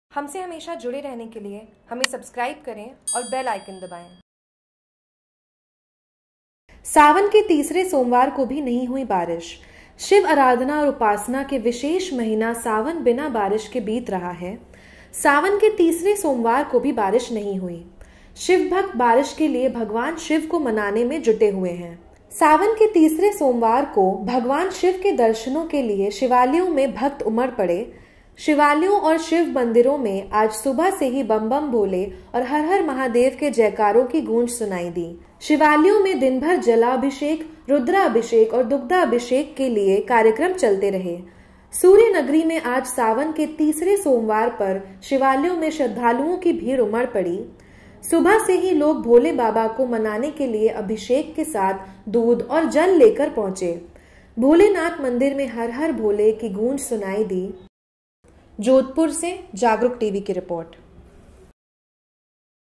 JAGRUK TV : जोधपुर। सावन के तीसरे सोमवार को भी नहीं हुई बारिश
जोधपुर। शिव आराधना और उपासना का विशेष महीना सावन बिना बारिश के बीत रहा है। सावन के तीसरे सोमवार को भी बारिश नहीं हुई। शिवभक्त बारिश के लिए भगवान शिव को मनाने में जुटे हुए है। सावन के तीसरे सोमवार को भगवान शिव के दर्शनों के लिए शिवालयों में भक्त उमड़ पड़े।